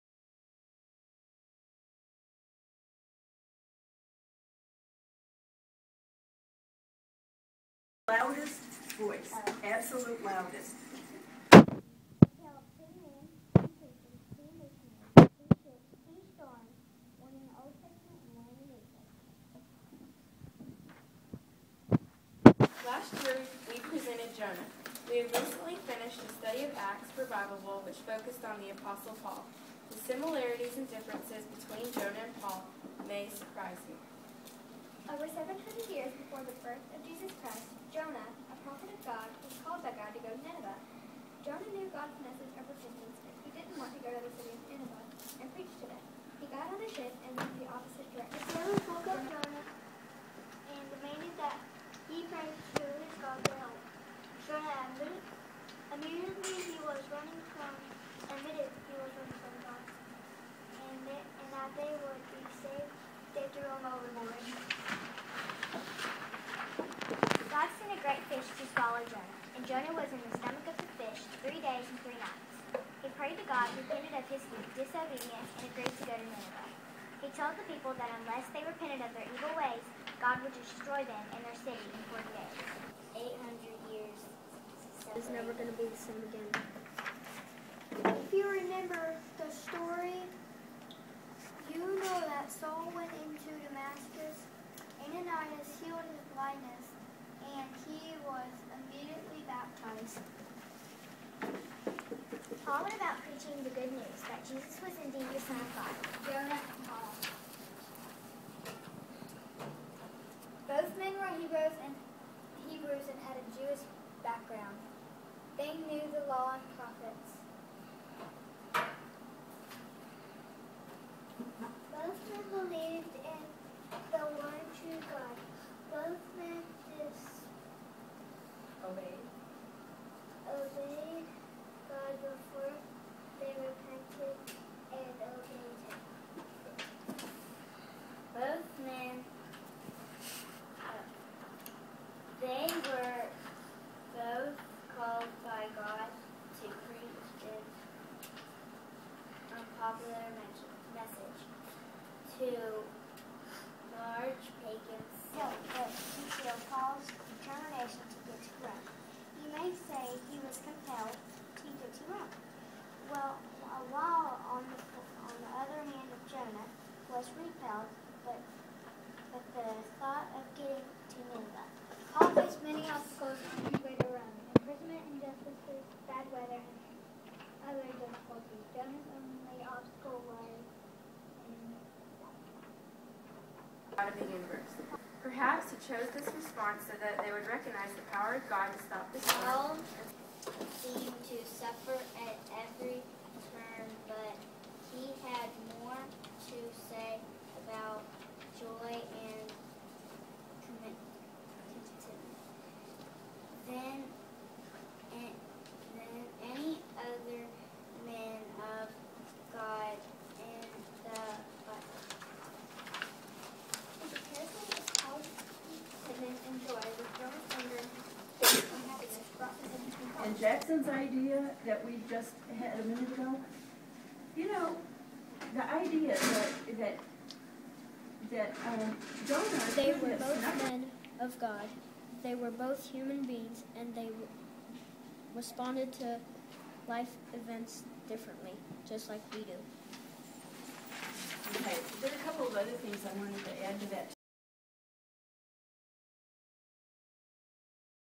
Paul and Jonah
Church kids explaining similarities and differences between Paul and Jonah.